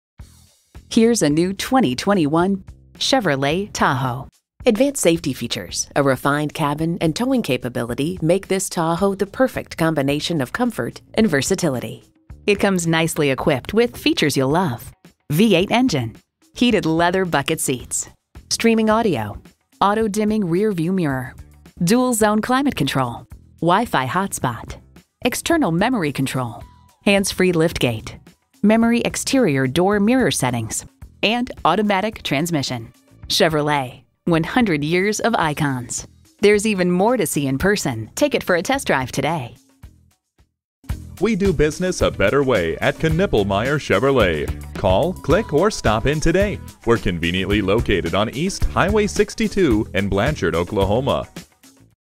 New 2021 Chevrolet Tahoe Oklahoma City Norman, OK #44090
Year: 2021
Make: Chevrolet
Model: Tahoe
Trim: LT 4WD
Engine: 5.3L V8
Transmission: Automatic 10-Speed
Color: Graywood Metallic
Interior: Jet Black w/Leather Seat Trim
Stock #: 44090
VIN: 1GNSKNKD8MR116152

Address:
1811 E. U.S. 62 Hwy